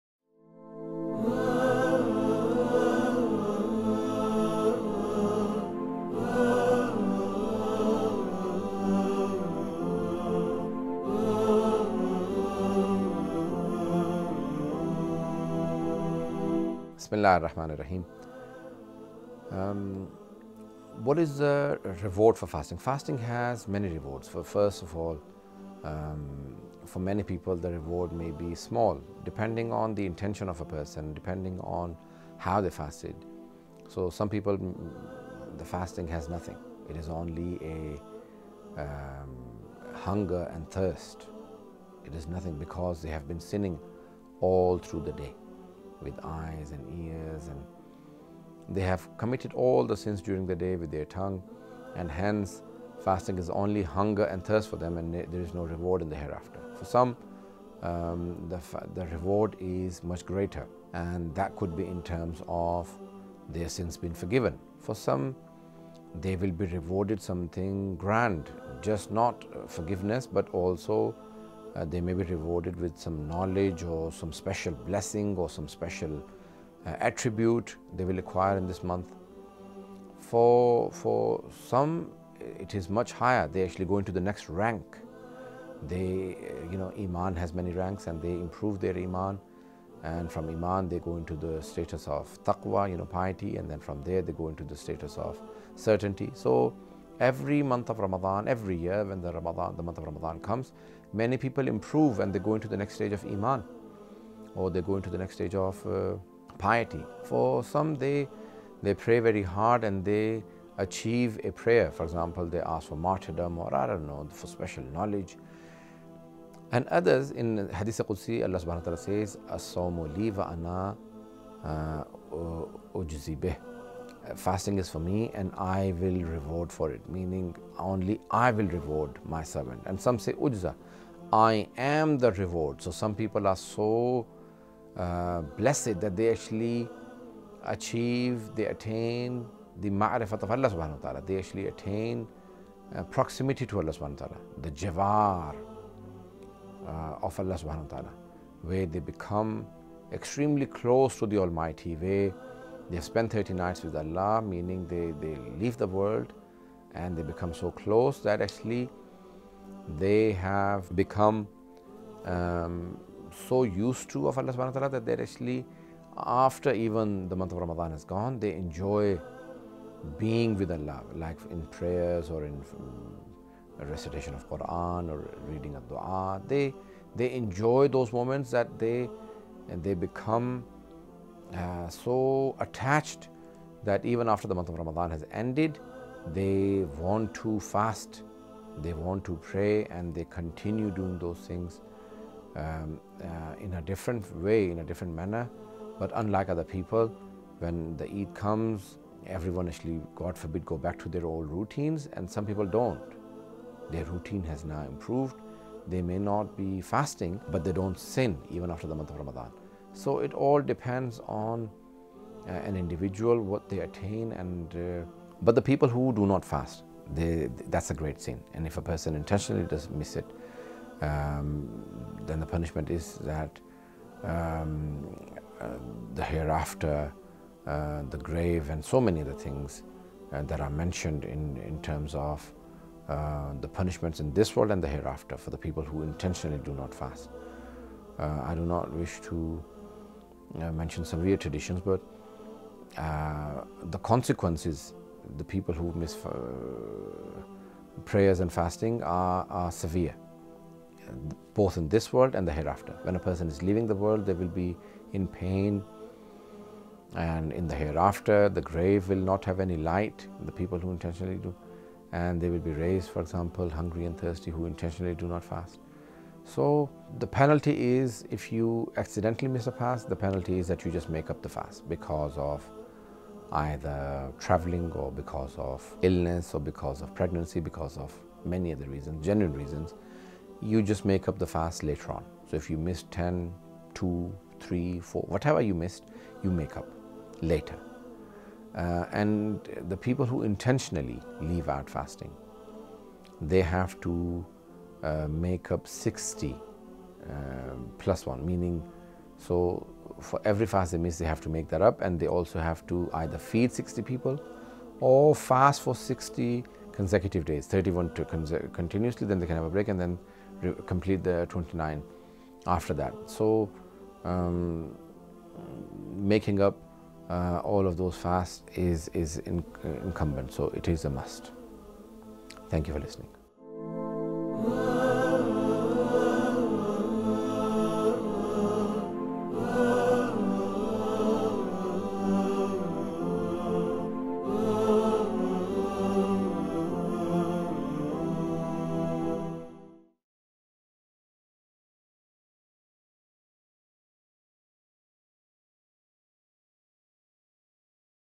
Rewards and punishments related to fasting - Syed Ali Raza Rizvi
Syed Ali Raza Rizvi draws out the significance and rewards for fasting. Is there any kind of punishment for people who intentionally do not fast in the Holy month of Ramadhan.